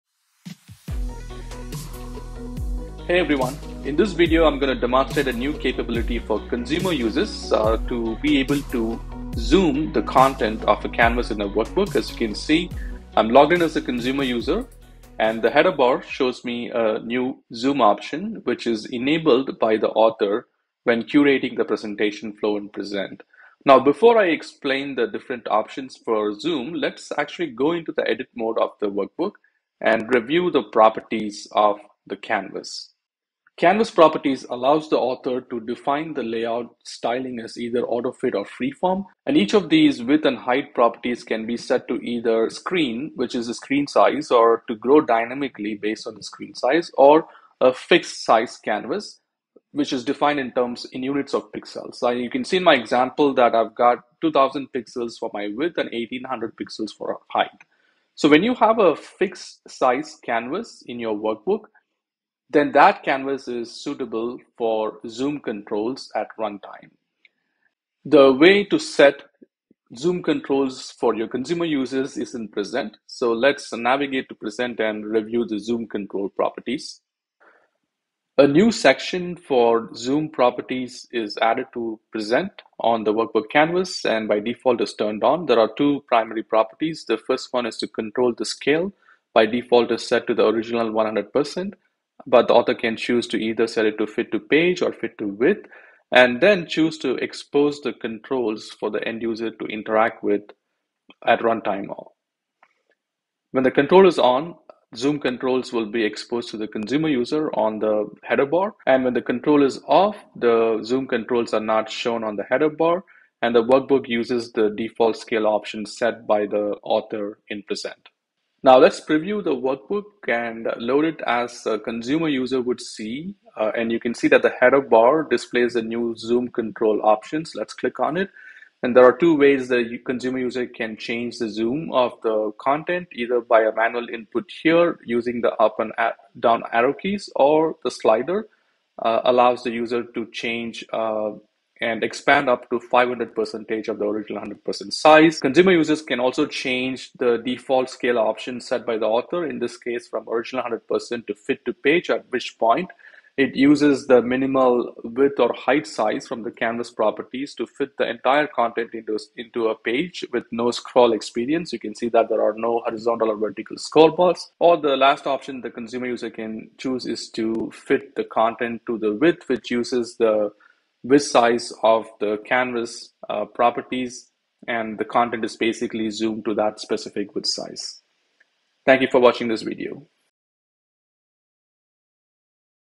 Enabling Workbook Zoom Controls in Oracle Analytics
This video shows how to enable canvas zoom controls in Oracle Analytics Data-Flows, matching.

A playlist of more of the new features that came to OAC in January 2024 is available